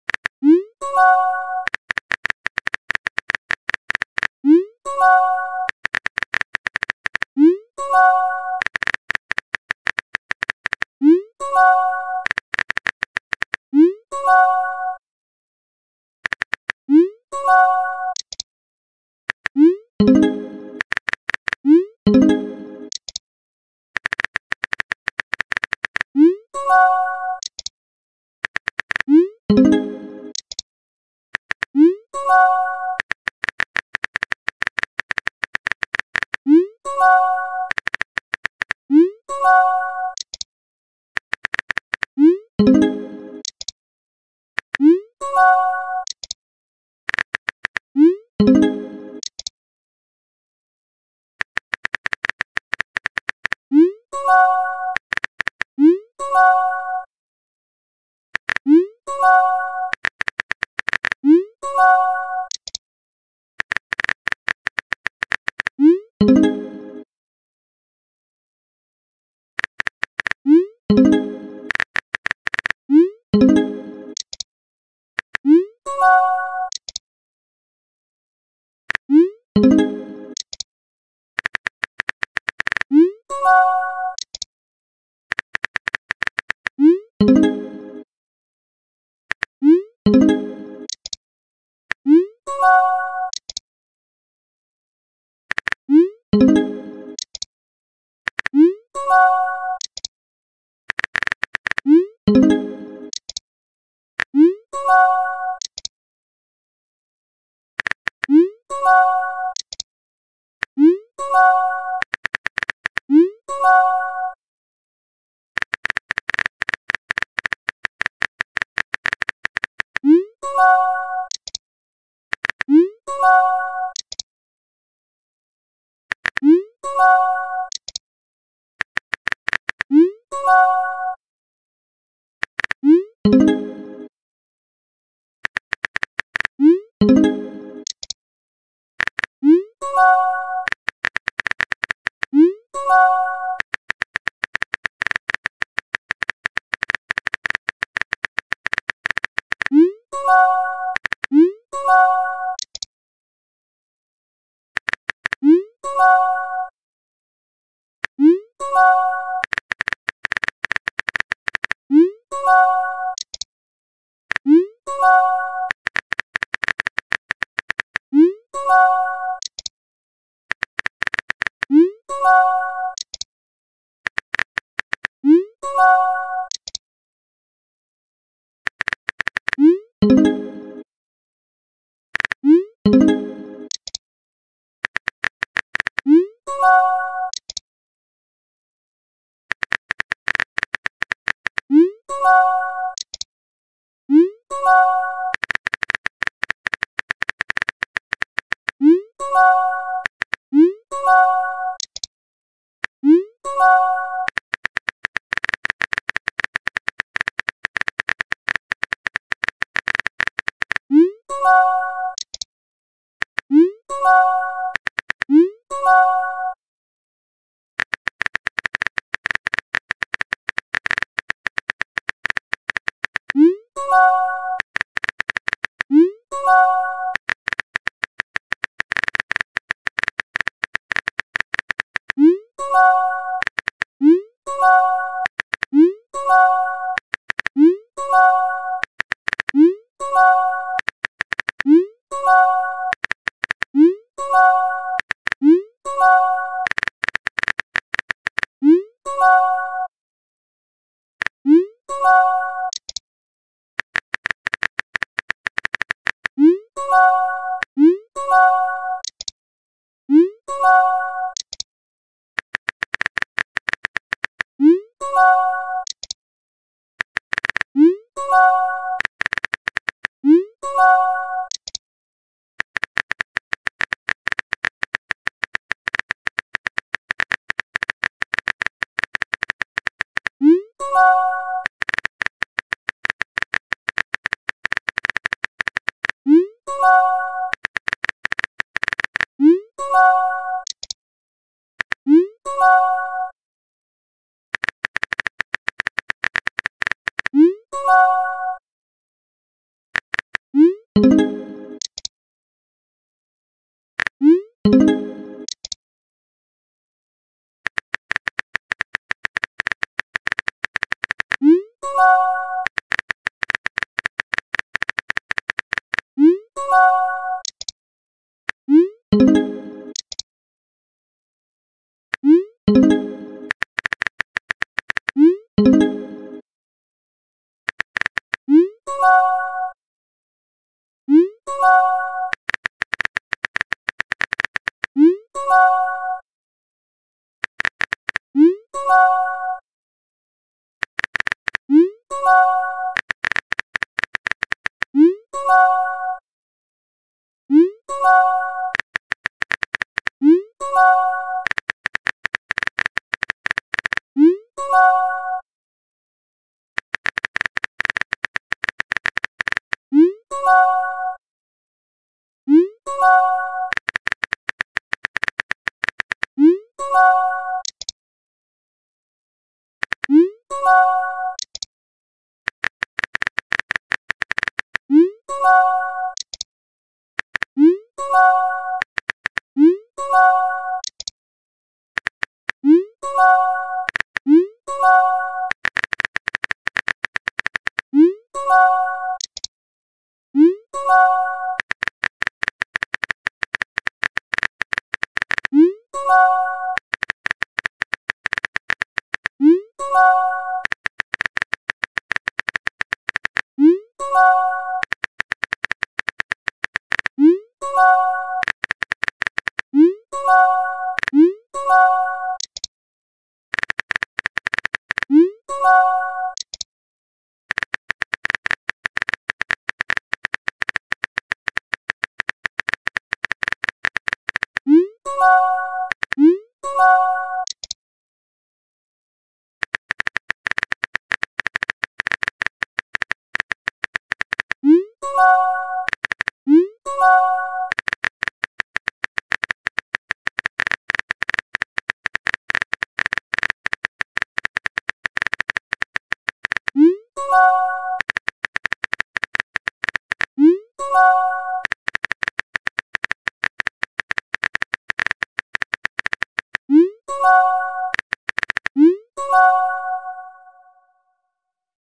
The hotel...
this is a haunted hotel that toxen, levi, and moon goes to. she chases a cat and then a murderer comes and does his job.